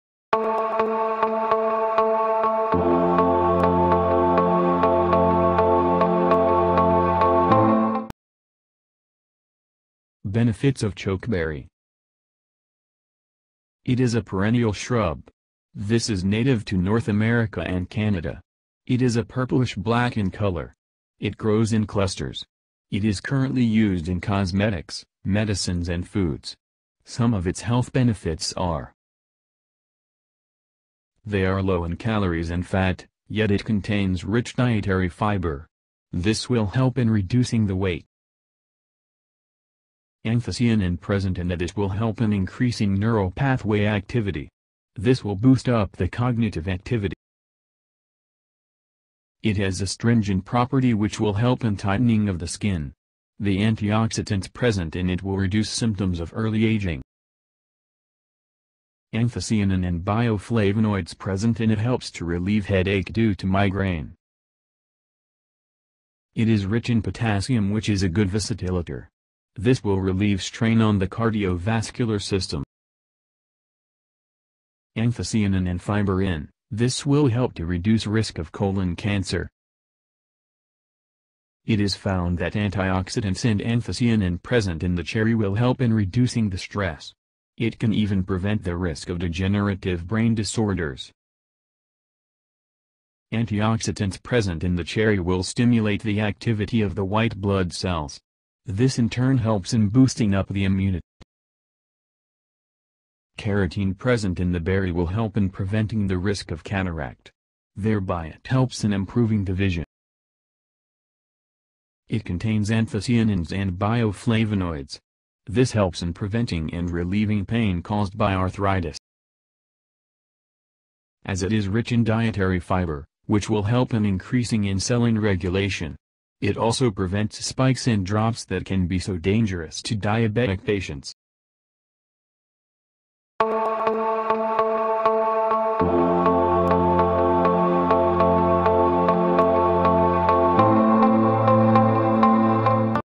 11 Health Benefits of Chokeberry
It is a perennial shrub. This is native to North America and Canada. It is a purplish black in color. It grows in clusters. It is currently used in cosmetics, medicines and foods. Some of its health benefits are.
1) They are low in calories and fat, yet it contains rich dietary fiber. This will help in reducing the weight.
2) Anthocyanin present in the this will help in increasing neural pathway activity. This will boost up the cognitive activity.
3) It has astringent property which will help in tightening of the skin. The antioxidants present in it will reduce symptoms of early aging.
4) Anthocyanin and bioflavonoids present in it helps to relieve headache due to migraine.
5) It is rich in potassium which is a good vasodilator . This will relieve strain on the cardiovascular system.
6) Anthocyanin and fiber in, this will help to reduce risk of colon cancer.
7) It is found that anti-oxidants and anthocyanin present in the cherry will help in reducing the stress. It can even prevent the risk of degenerative brain disorders.
8) Antioxidants present in the cherry will stimulate the activity of the white blood cells. This in turn helps in boosting up the immunity.
9) Carotene present in the berry will help in preventing the risk of cataract. There by it helps in improving the vision.
10) It contains anthocyanins and bioflavonoids. This helps in preventing and relieving pain caused by arthritis.
11) As it is rich in dietary fiber, which will help in increasing insulin regulation. It also prevents spikes and drops that can be so dangerous to diabetic patients.
Meet Dr. Purushothaman@Centre For Human Perfection:
About Dr. Purushothaman:
Dr.Purushothaman'sBooks @ Amazon.com
Living In Wellbeing Books@ Kindle